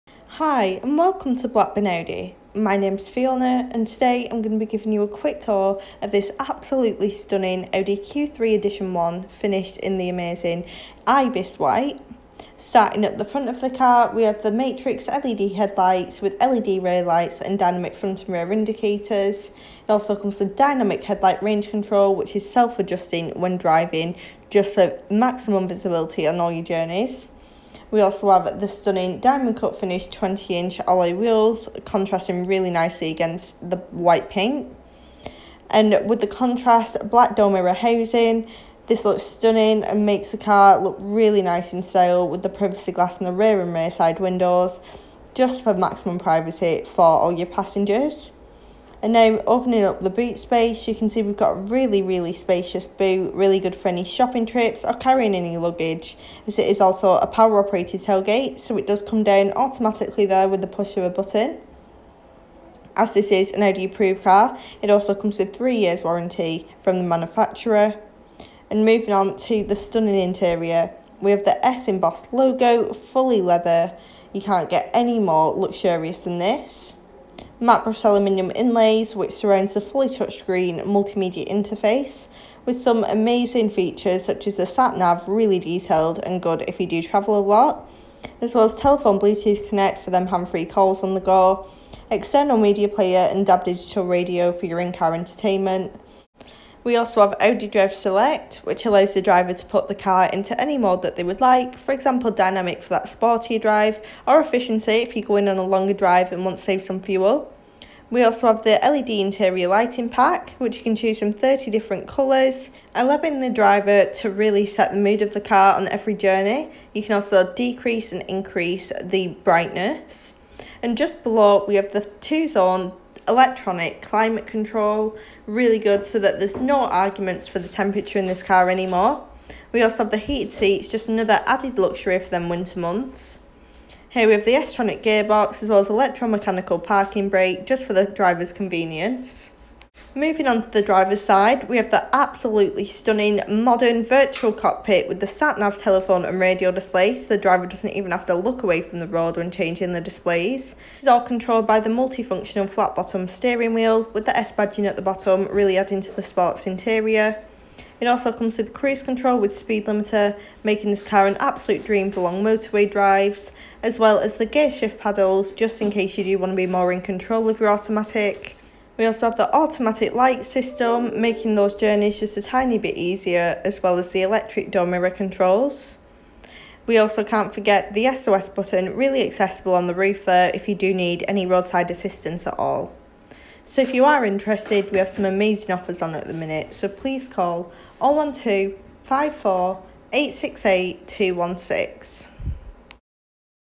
Audi Approved Q3 Edition 1 1.5 Petrol Automatic | Blackburn Audi
Just into our stock, this Audi Q3 in Ibis White, is now available for sale from Blackburn Audi.

Reg: PK21 UOX
Fuel: Petrol
Transmission: Automatic
Engine Size: 1.5
Mileage: 3,000 miles

Features include:
Twin leather upholstery
Matrix LED headlights with LED rear lights and dynamic front and rear indicators
Black Styling Pack
Headlight washer system
Audi Virtual Cockpit Plus